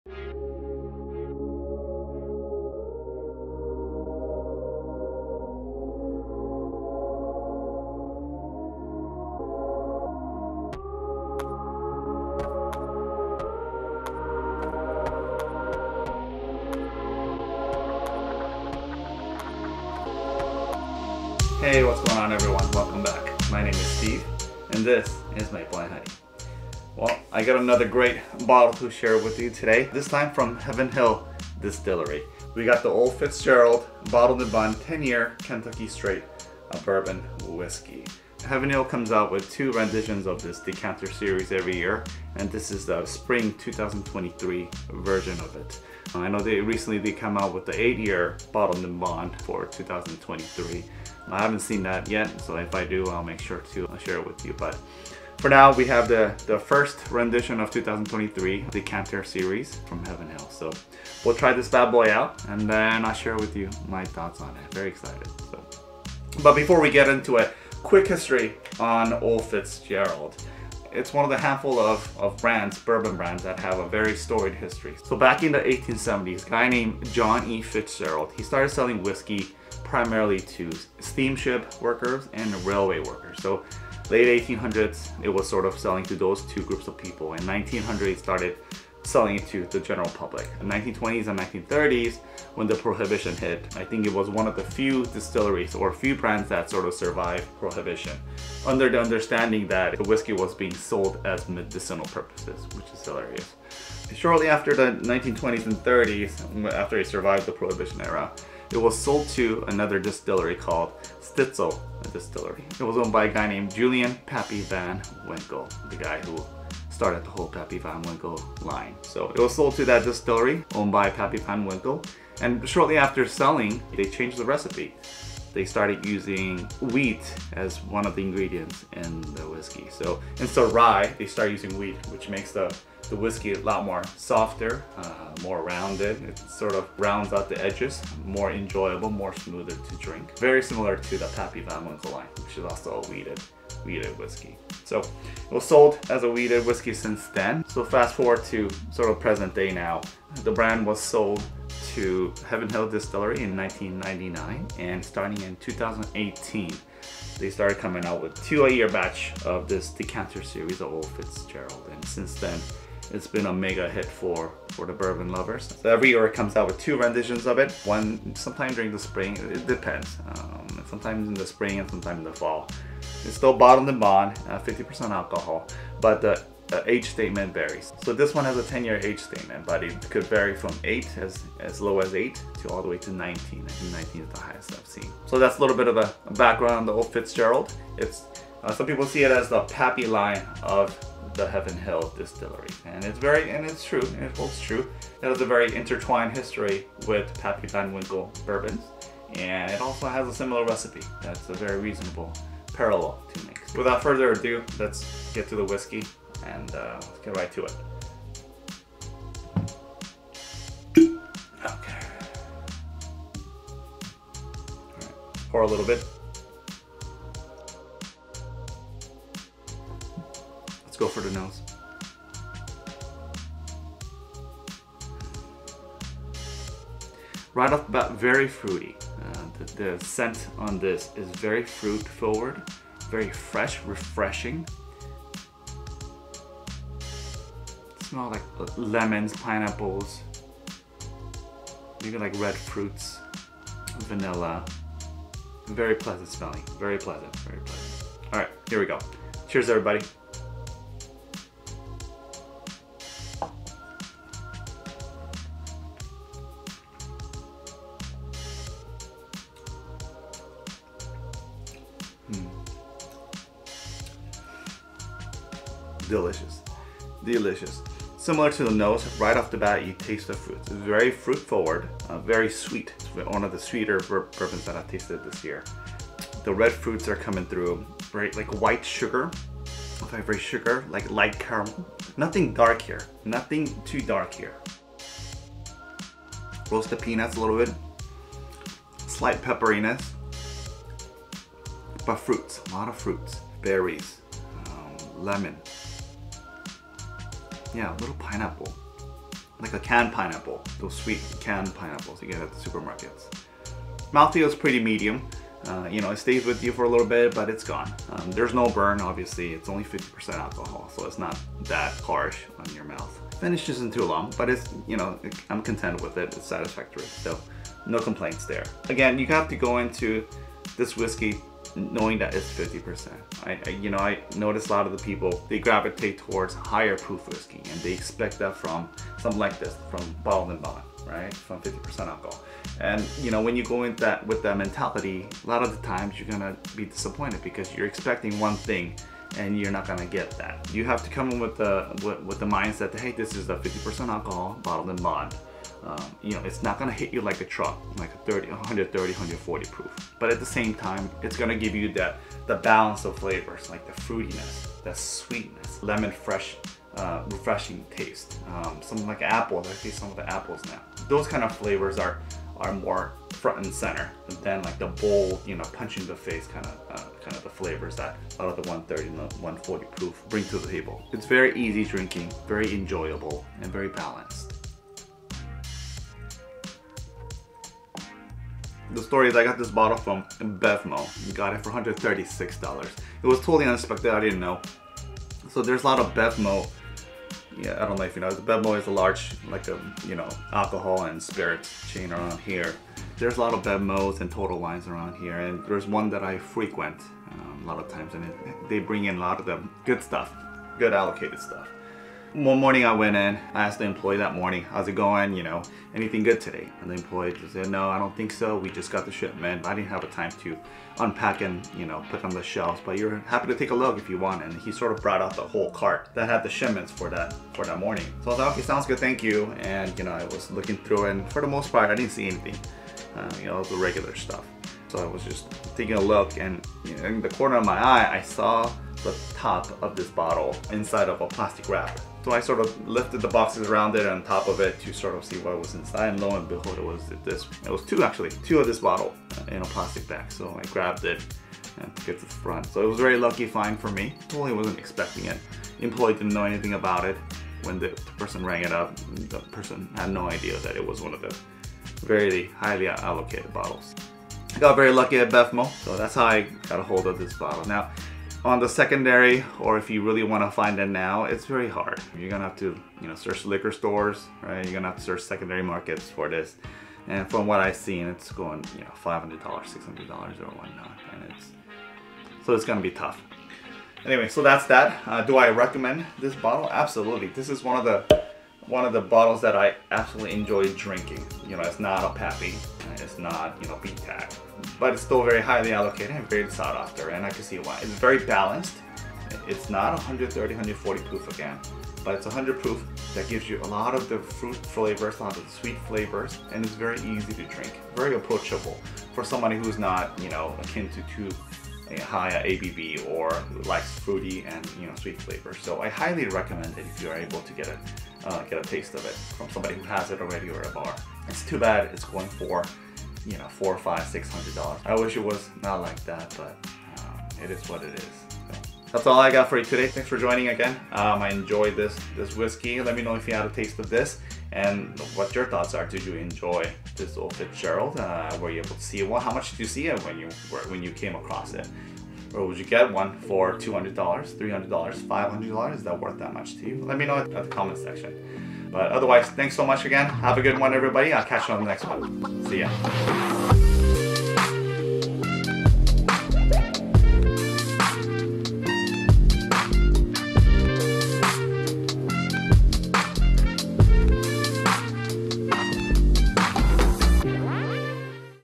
Decanter Delight: Old Fitzgerald 10 Year Bourbon Review - Sought-After Beauty of 2023!
Unveil the allure of Old Fitzgerald Bourbon in 'Decanter Delight: Old Fitzgerald Bourbon Review - Sought-After Beauty!' This delicious brown juice, encased in a stunning glass decanter, is a whiskey lover's dream.